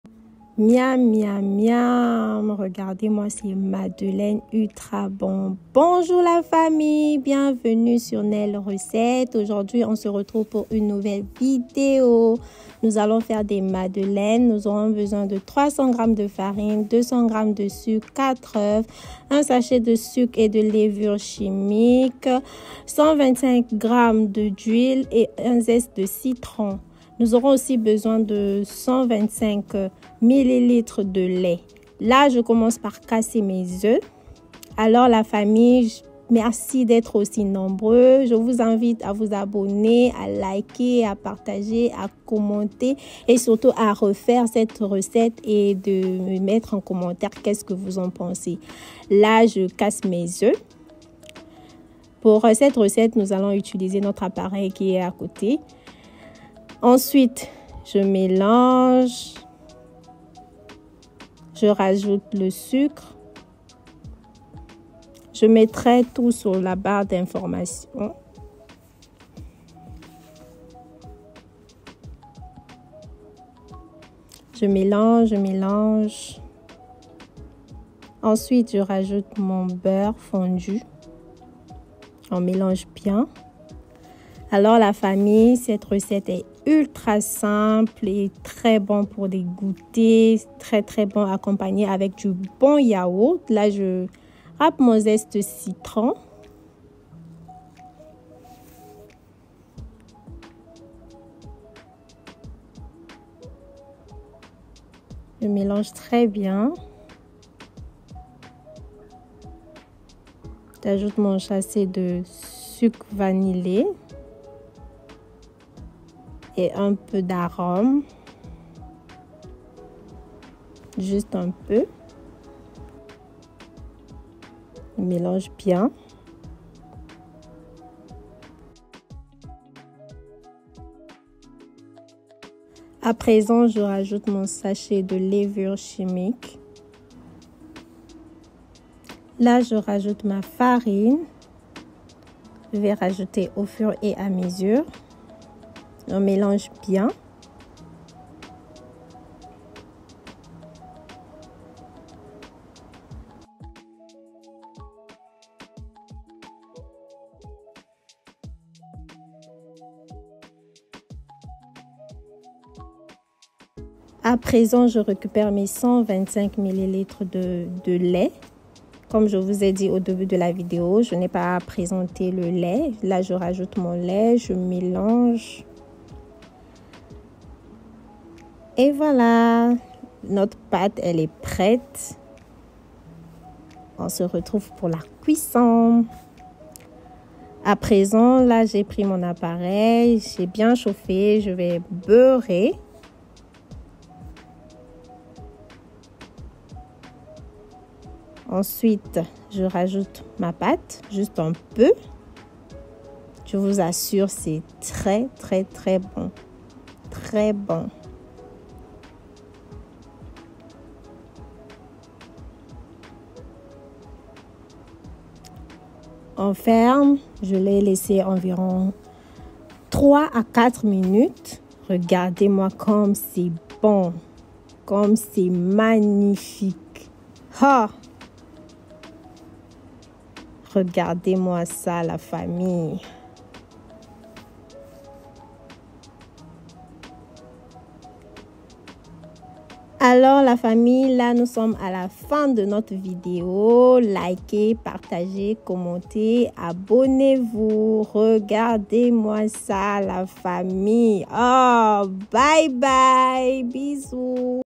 madeleine ultra simple et très bon😋😋😋
300g de farine
200g de sucre
1 sachet de sucre vanillé
1 sachet de levûre chimique
4 œufs
125ml de lait
125g d’huile